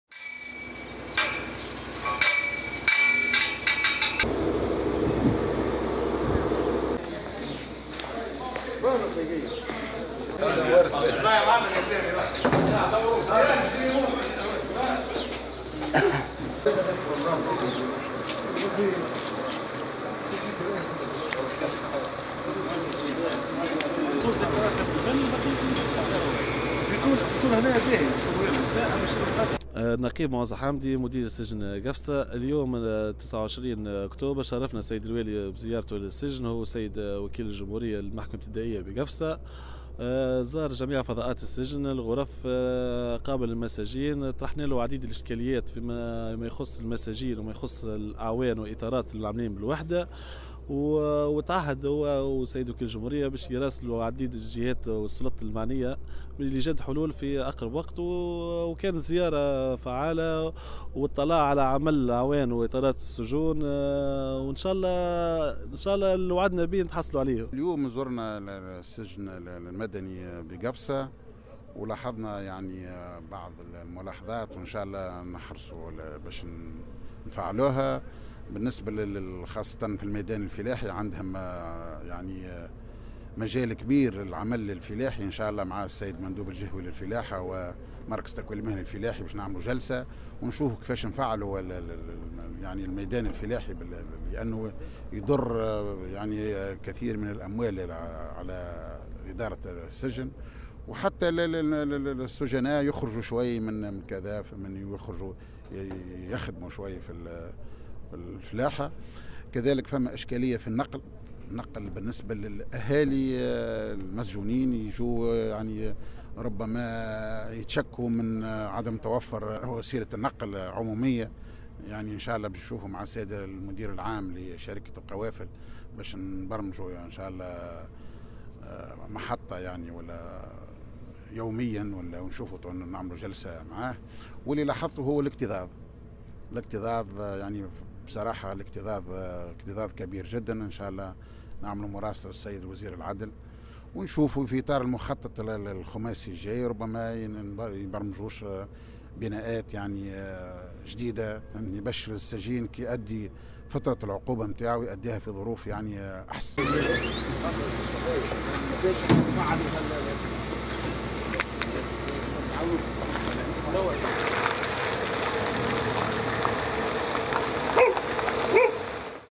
قفصة:زيارة تفقدية للسجن المدني من قبل والي الجهة
ادى والي قفصة السيد الطيب الزارعي صباح اليوم زيارة الى السجن المدني بقفصة اطلع خلالها على وضعية المساجين و وضعية المؤسسة السجنية هذا وقد صرح والي الجهة ان الوحدة السجنية تشكو من بعض النقائص التي وجب العمل على تجاوزها في اقرب الاجال مؤكدا كذلك انه سيتم مراسلة الجهات المسؤولة للعمل على الحد من بعض الاشكاليات.